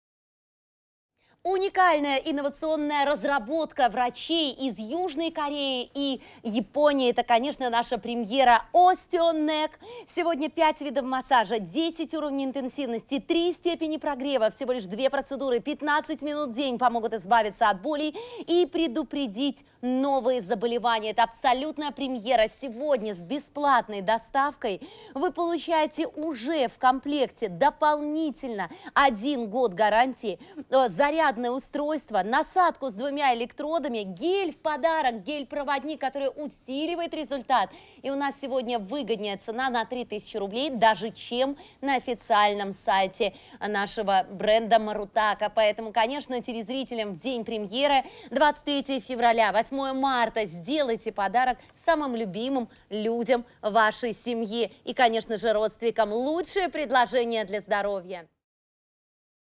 Массажер для шеи Marutaka Osteo Neck при остеохондрозе. Shop & Show (Здоровье)
[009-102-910] Массажер для шеи Marutaka Osteo Neck при остеохондрозе + подарок (гель-проводник, 30мл) (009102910)

Давайте дружить ;)